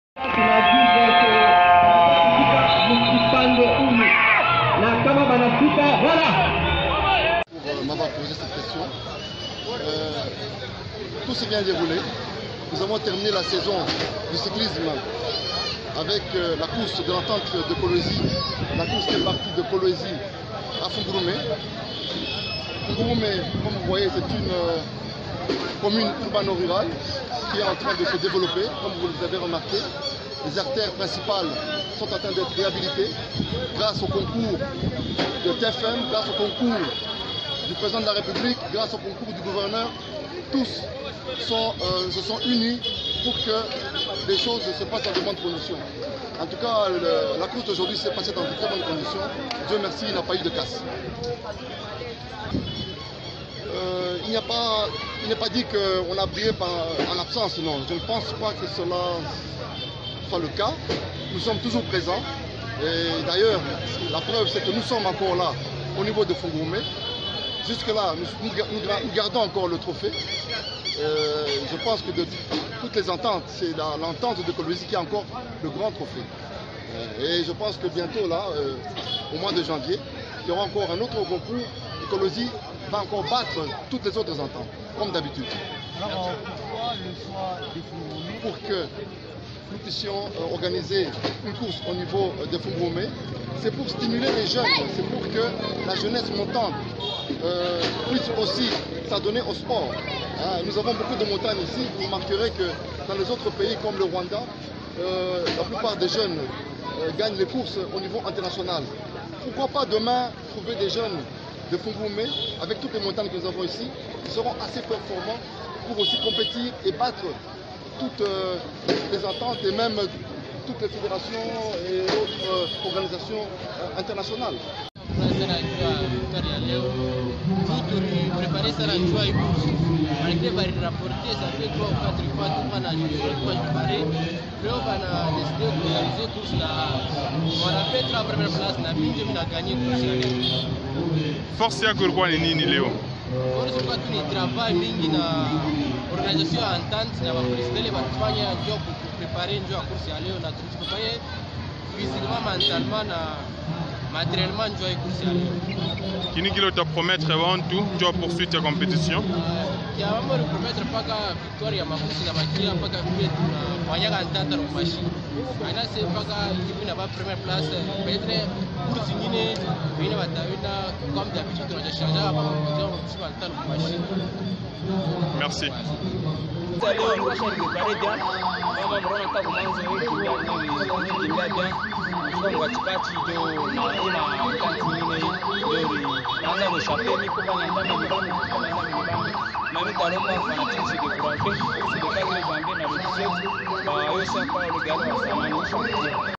CLOTURE DE LA SAISON A L'ENTENTE DE KOLWEZI,DU TOUR CYCLISTES
Après 110 kilomètres Kolwezi fungurume   pour la clôture de la saison de l'Entente de Kolwezi , le classement du podium se présente comme suite : 1. Martino mbiya(de Lubumbashi)
2. Hussein yanda (de Lubumbashi)
3.Ali kamanga (de Lubumbashi)